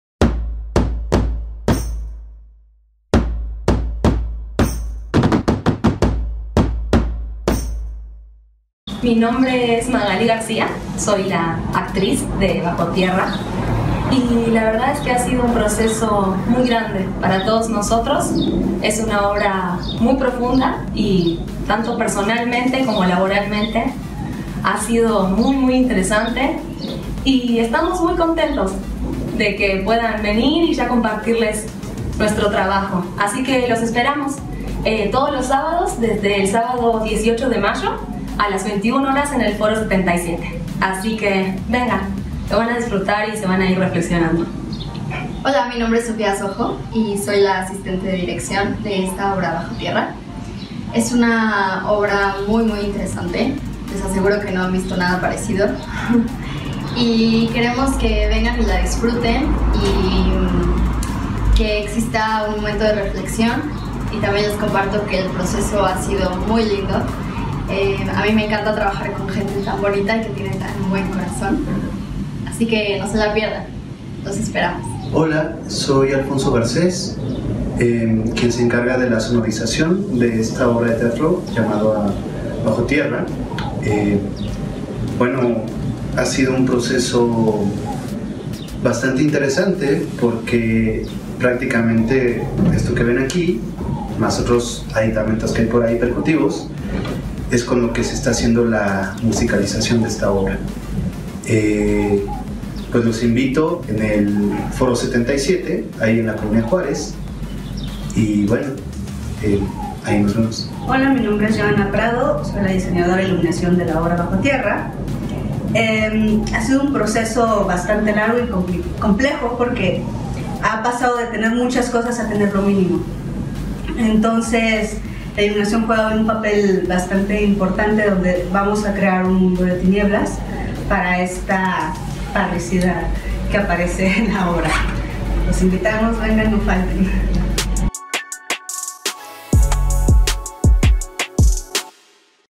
Obra de Teatro "Bajo Tierra" Foro 77 Imperdible
La recomendación 8yMedia es esta fantástica obra de Teatro, "Bajo Tierra", gran obra, fantásticas actuaciones y perfectamente bien realizada y dirigida. Foro 77 en la Col. Juárez